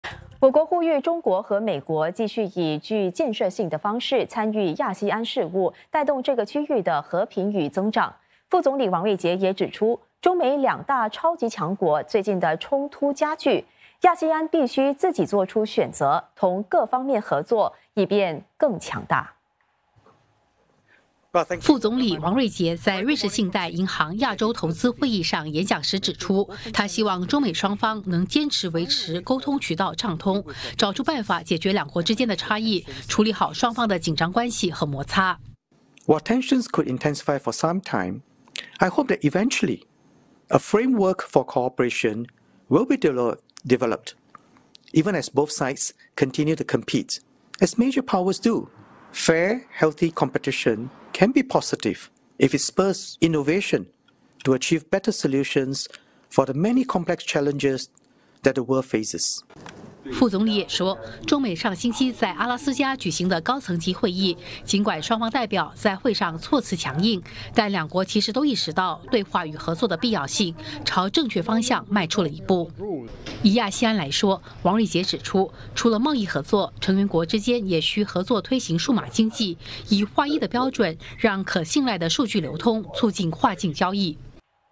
王瑞杰：中美相互竞争亦能合作 两国须开放沟通渠道
副总理王瑞杰表示，美国和中国上星期在阿拉斯加举行的会议显示，中美双方都意识到对话与合作的必要性。尽管两国在会上措辞强硬，但双方却向正确的方向迈出了一步。